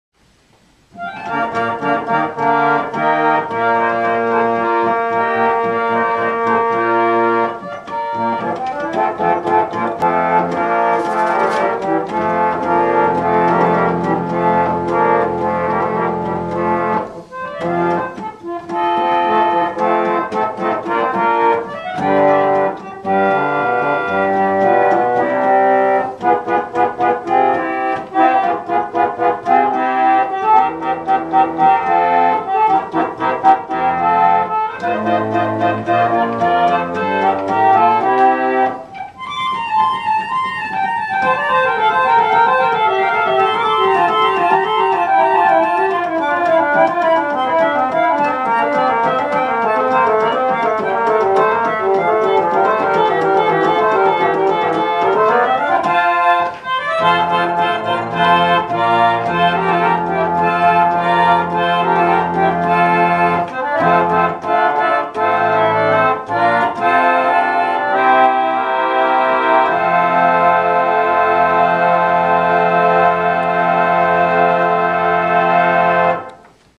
Martin Ellis on Indiana Humanities' reed organ
Thank you, Martin, for test driving our new reed organ!